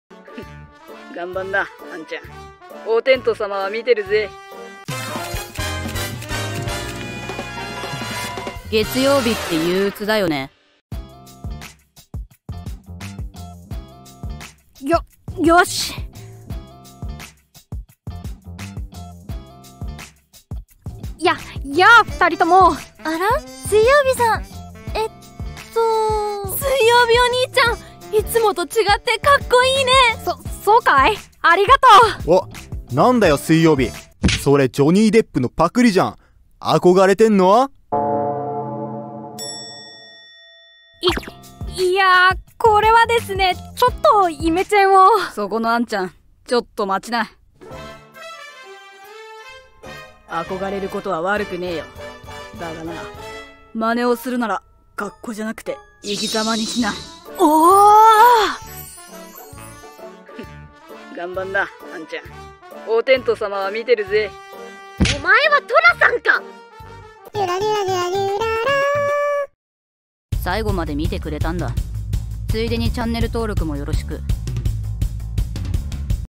【アニメ】某俳優に憧れて服装を真似してみた男子学生→友達「真似をするなら恰好ではなく生き様にしろ」→その一言で人生に転機が訪れる【漫画動画】
今日のマンガ動画は、とある有名な俳優に憧れを頂いた少年。一歩でも憧れの存在に近づくために服装を真似してみることに...。しかし周りの友人からは馬鹿にされてしまう。そんな中一人の友達が放った一言で転機が訪れる。

【声優】
ヒューマンアカデミー那覇校パフォーミングアーツカレッジ
月曜日ちゃん：大城美貴
火曜日ちゃん：米原優
水曜日君：榮門汐音
木曜日君：なーちゃん
金曜日君：福原兼寿
土曜日ちゃん：Mao♪
日曜日ちゃん：りん
その他キャラ参加者：カズ、遠藤希穂、當眞智也子、西山清香、仲程祭
【シナリオ】
瀬古英司